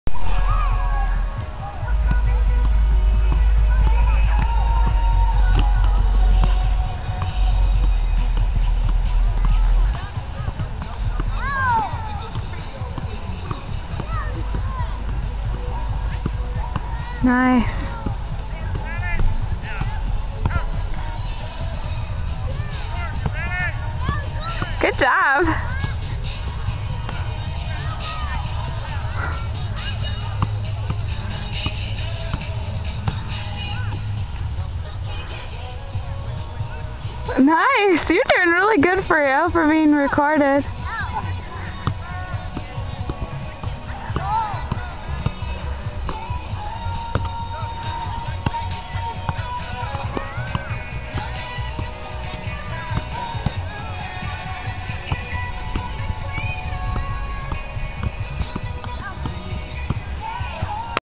Manuel Juggling the Soccer Ball
just a little bit of my friend's mad skills